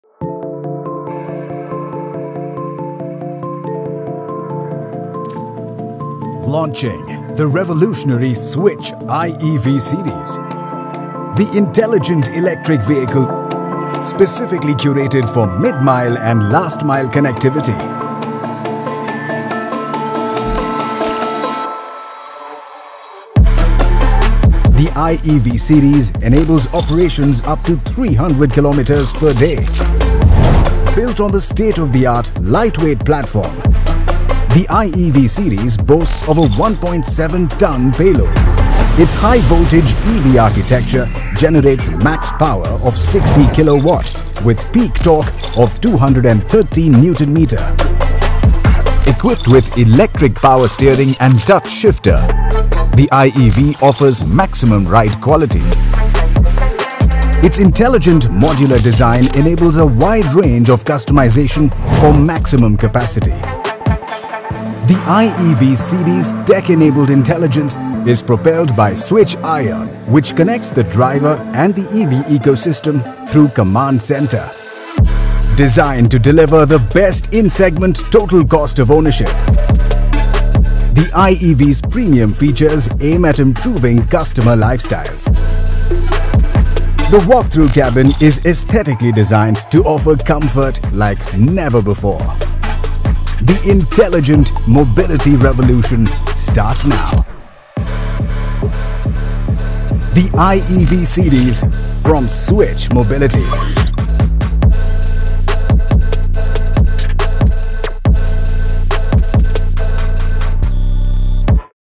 SWITCH IeV Series
The Switch IeV Series is a game-changer in the electric Light Commercial Vehicle (eLCV) segment, redefining mid and last-mile transportation. It showcases Switch's global expertise in electrifying logistics. With an industry-leading turnaround time, the Switch IeV is set to revolutionize last-mile logistics, paving the way for a more efficient and sustainable urban mobility future.